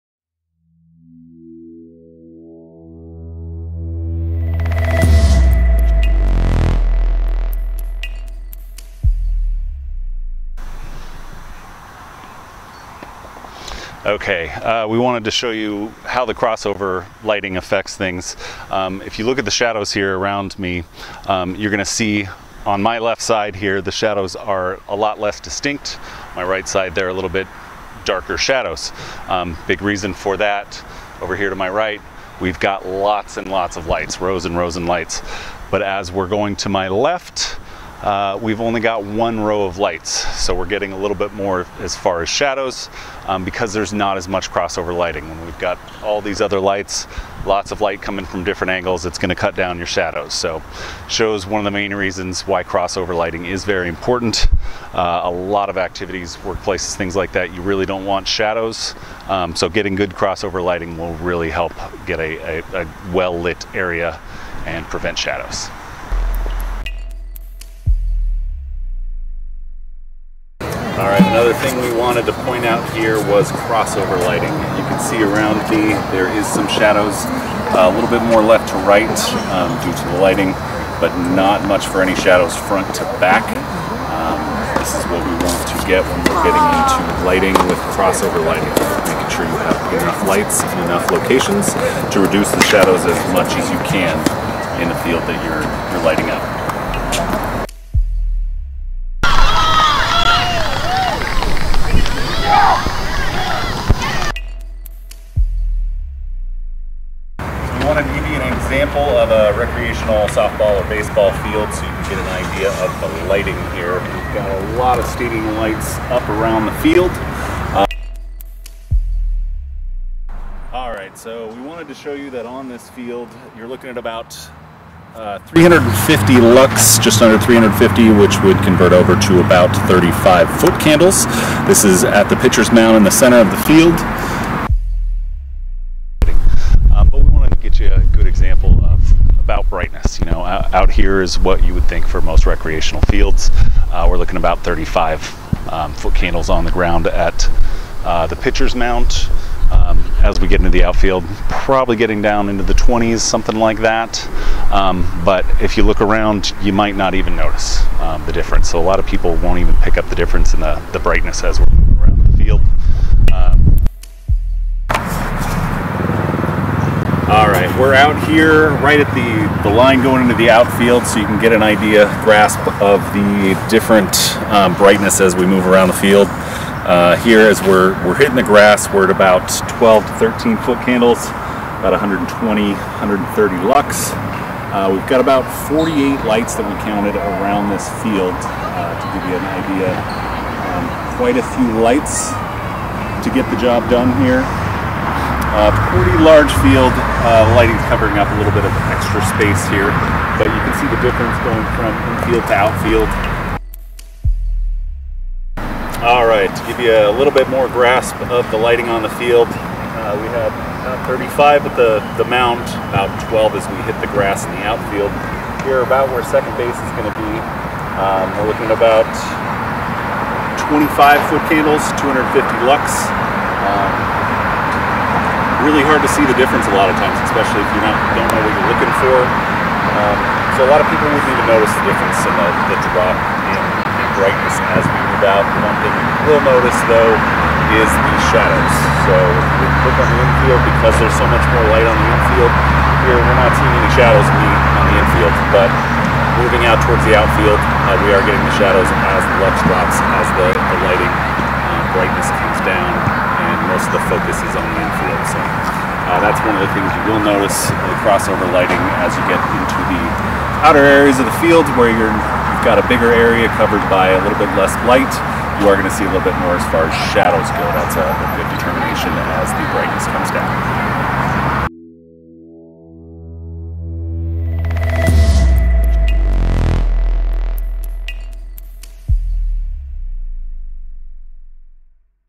What is Cross Over Lighting? Why is even lighting important? Cross Over Lighting LED Sports Lights
What is Cross Over Lighting?  Why is it important?   Cross Over Lighting for LED Sports Lights

Getting to 50 footcandles is great, but not if its 1 bright light.   That creates too much shadow, and inconsistency.   We design lighting with luminaries (light fixtures) in multiple locations to create what is known as 'crossover' lighting.   This is light coming from multiple directions to help reduce shadows, bright spots and general evenness.  We typically recommend 4 lights at a minimum because that is cross over lighting from 2 different directions.

Good cross over lighting also reduces glare, which is critical in sports applications.    Even light reduces the looking at the sun affect which is better for the eyes.

- Crossover lighting on a tennis court
- Baseball Field Lighting
- importance of cross over lighting
- Softball field lighting
- footcandle levels at various points of light